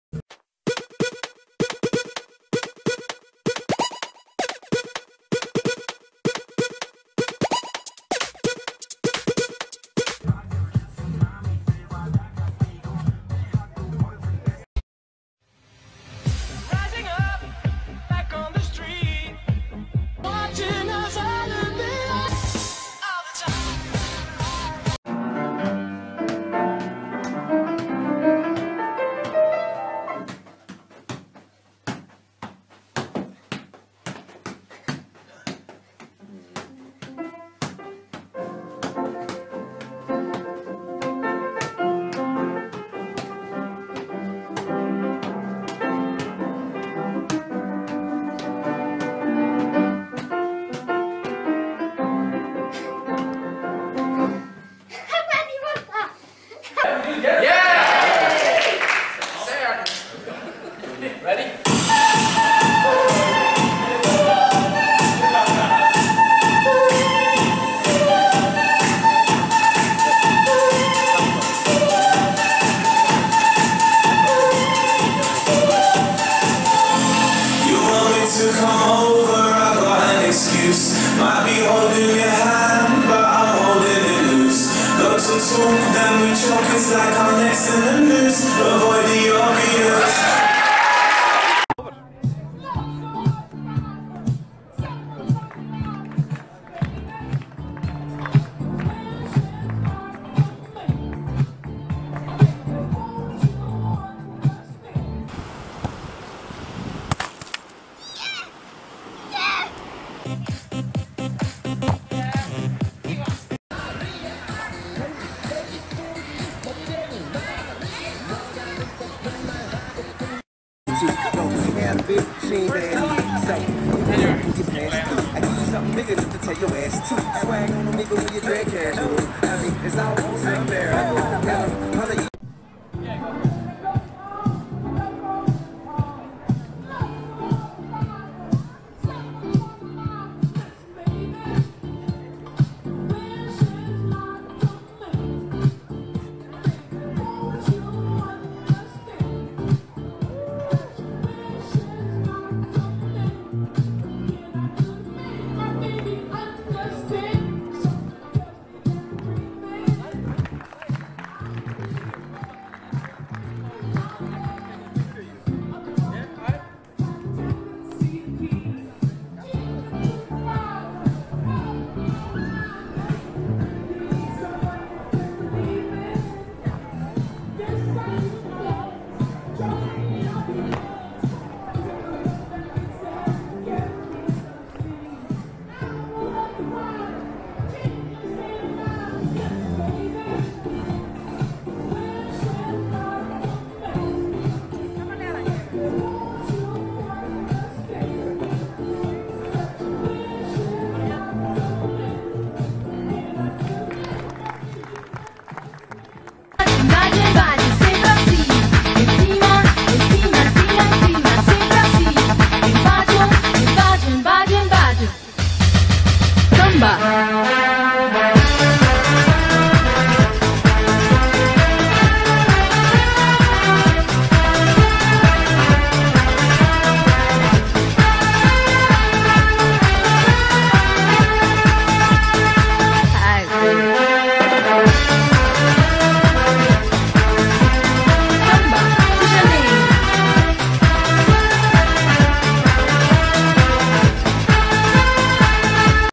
Youth Soccer Ball Skills - Mini Messi Mix Up
A mix up of various clips, shots, street performing, show performances. On Astro Grass, Two V Two, Rugby Ball Juggling.
Juggling down probably the Steepest Sand Dune in Ireland which is particularly challenging to control the ball!
Thanks for looking. There will be new stuff soon, very busy with Soccer training everyday at the moment but love that.

Check out the World's biggest collection of original football shirts